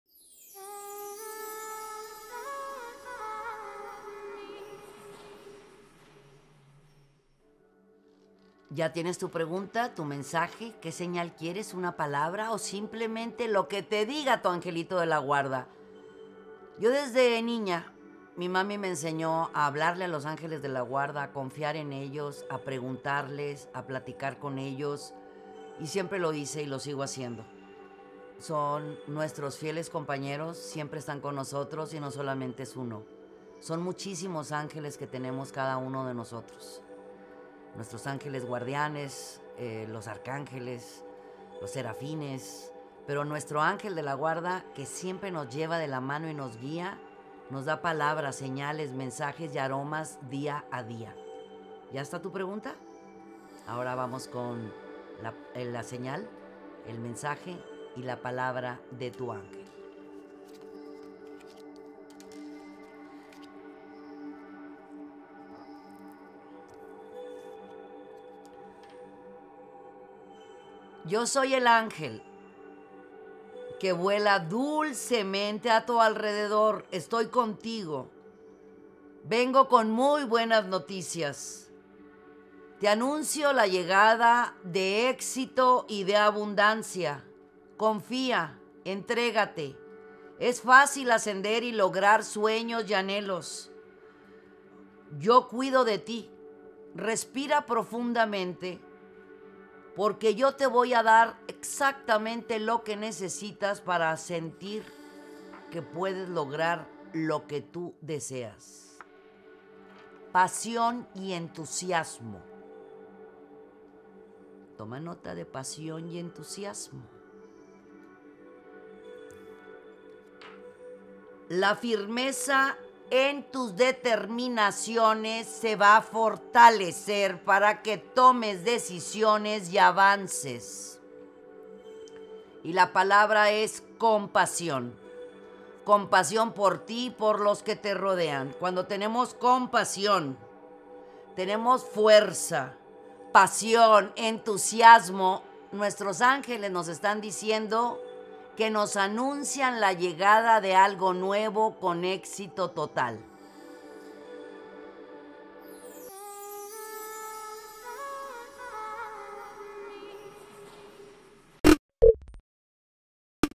Mensaje de tu Ángel. Segunda Semana de Febrero 2022. Mizada Mohamed.
🌟 Conoce el Mensaje de tu Ángel que Mizada Mohamed tiene para ti para la segunda semana de febrero 2022. Solo haz tu pregunta y escucha con atención.